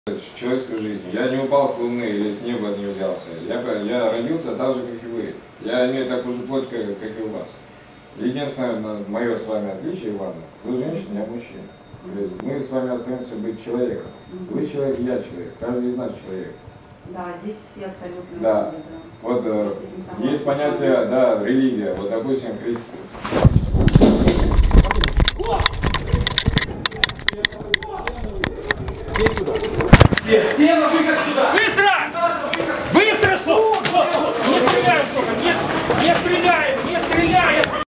Захват террориста в БЦ "Леонардо"
UA - відео з камери оперативника і процес знешкодження терориста.

RU - видео с камеры оперативника и процесс обезвреживания террориста.